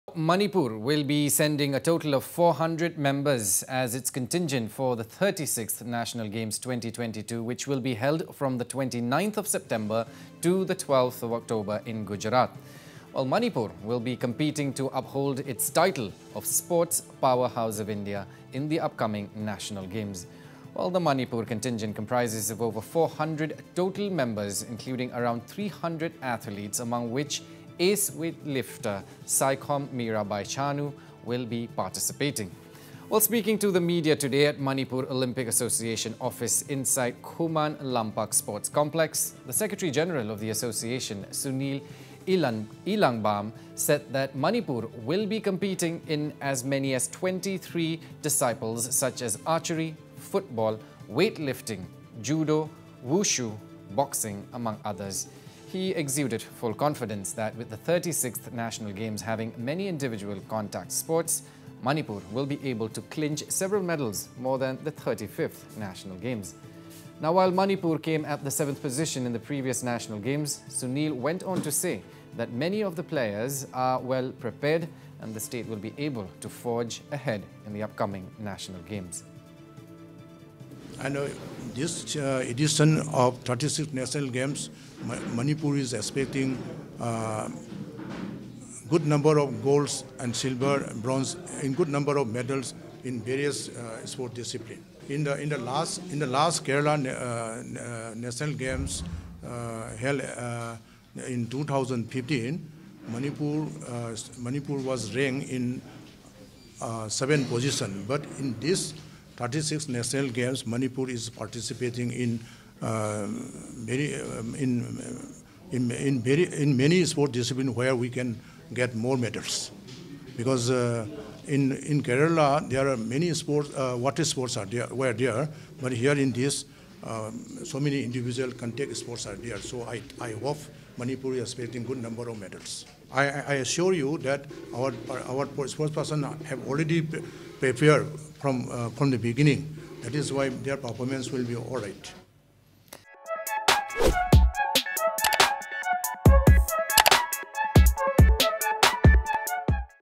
Manipur to send 300 athletes to 36th National Games, 2022 in Gujarat
Manipur will be sending a total of 400 members as its contingent for the 36th National Games, 2022 which will be held from 29th September to 12th October in Gujarat. Manipur will be competing to uphold its title of ‘Sports powerhouse of India ' in the upcoming National Games. The Manipur contingent comprises of over 400 total members including around 300 athletes among which ace weightlifter Saikhom Mirabai Chanu will be participating.